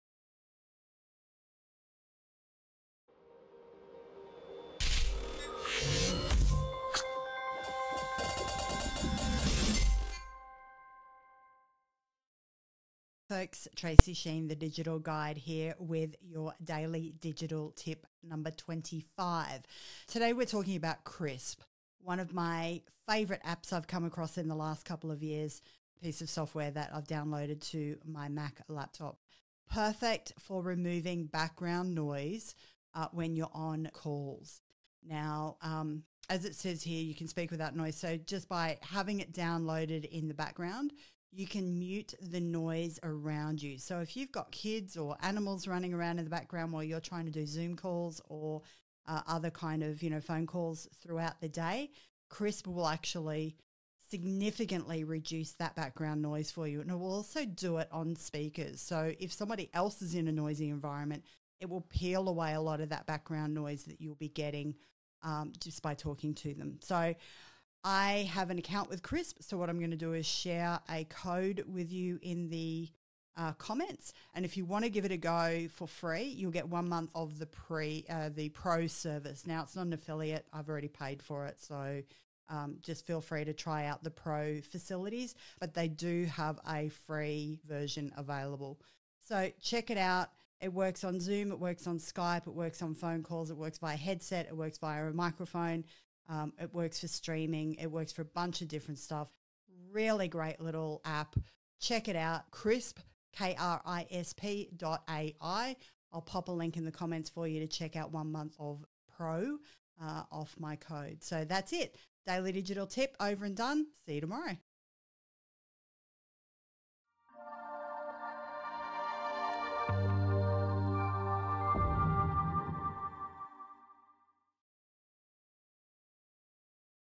Tech Tips for Small Business - Krisp
Sick of kids screaming, dogs barking and lawnmowers going off when you're trying to do a Zoom call (or record a podcast)?

Today I'm going to share an app that's an absolute game changer...
Introducing Krisp

COOL STUFF YOU NEED TO KNOW!!!